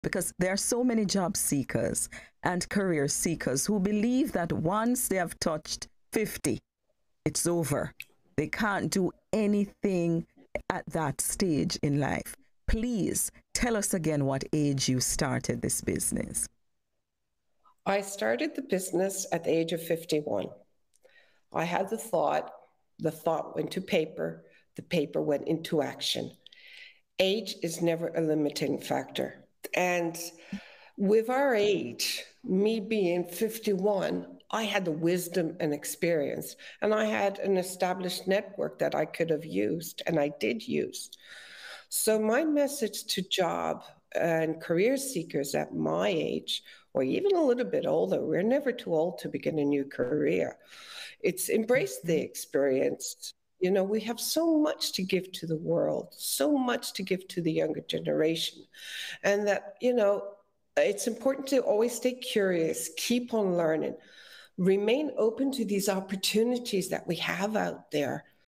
Age is Just a Number!
Age is Just a Number to Begin a New Career or Start a Business! Watch the conversation with Megan Wade & Wedding Planner - Maria Viera